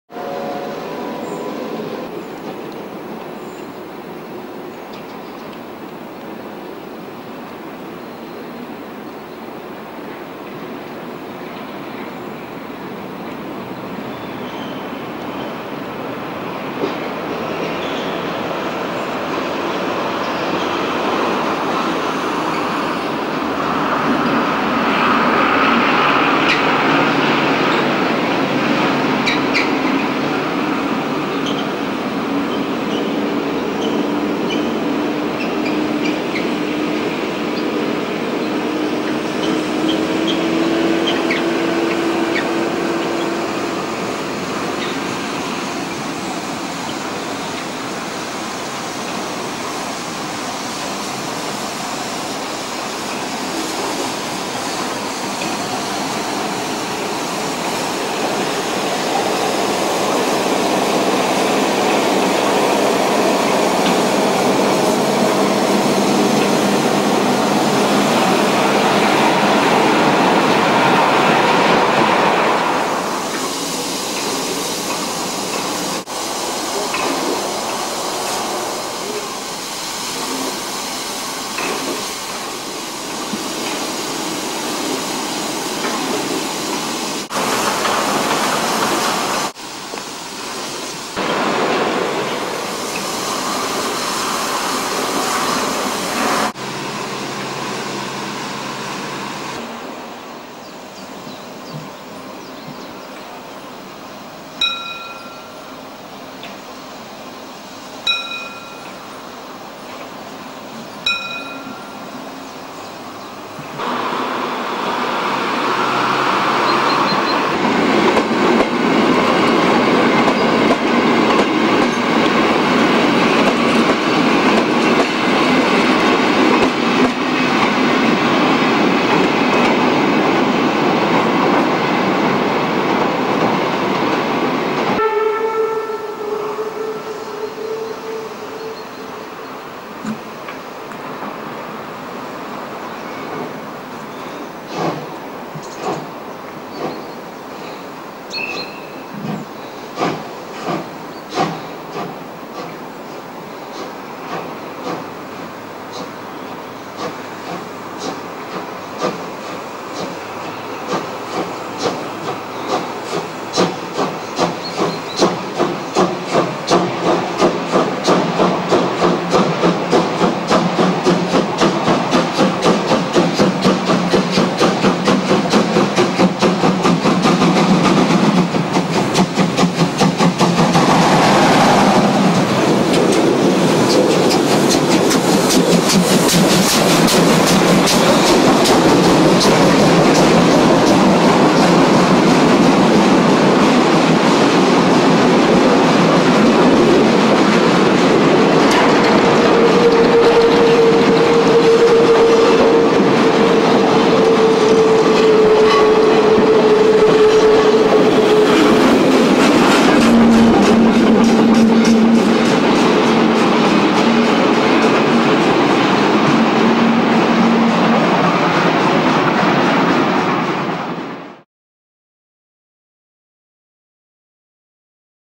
DampfLok 50 3646-2 Dittersdorf DDR 1987 Teil 2
Dittersdorf Ankunft um 08u30 von Dampflok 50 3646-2  mit ein Güterzug und abfahrt nach Aue.Und ankunft Personen Zug aus Aue. gezogen durch ein Lok der Baureihe 110 ,am 16ste September 1987